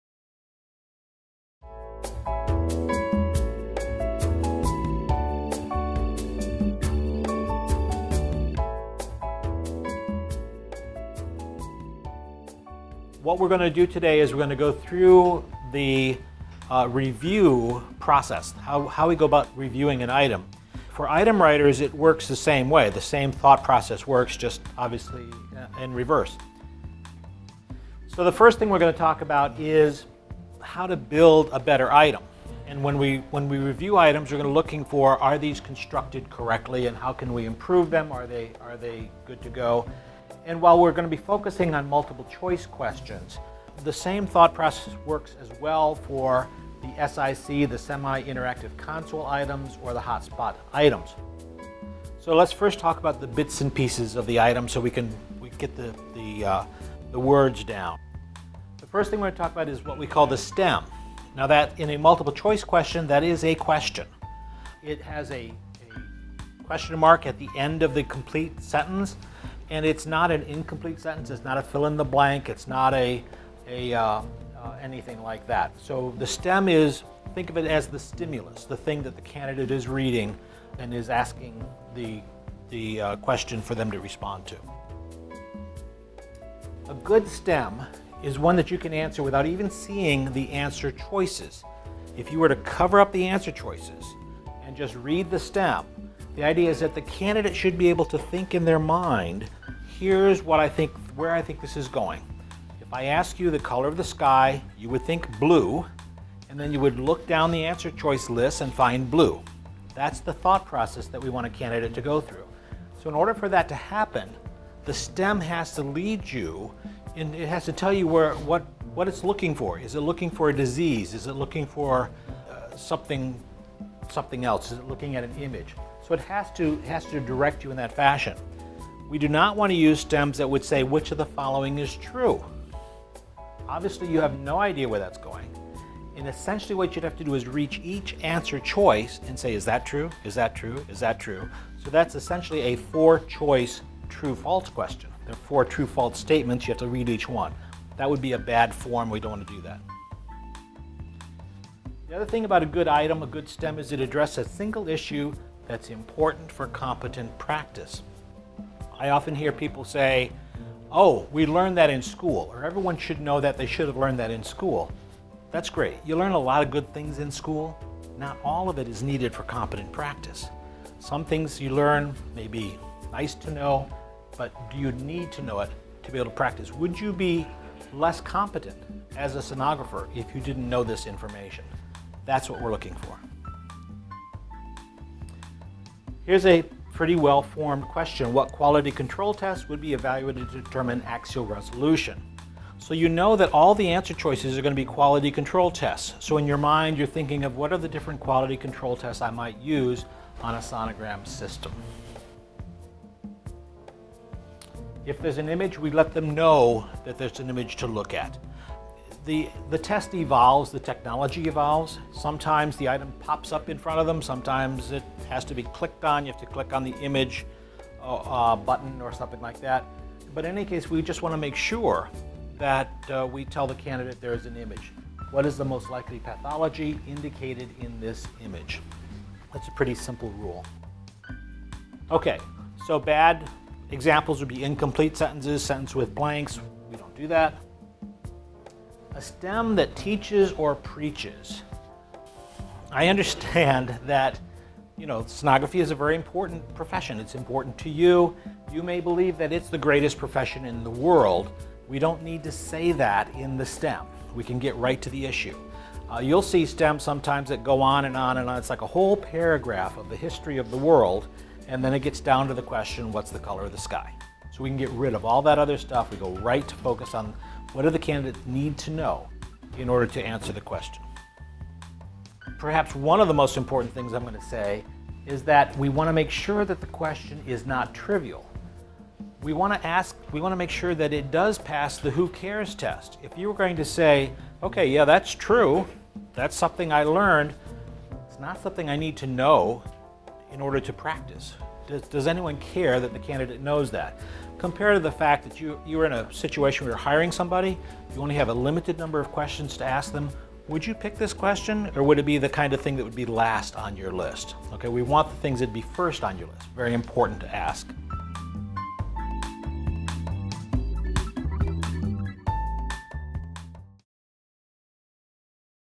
01 How to Write an Item
Part 1 of 3 (pre-IDW briefing)

The ARDMS how to write an item (test question). This is a primer for the Item Development Workshops (IDW's) and is meant to assist our volunteers in preparing for upcoming IDW's.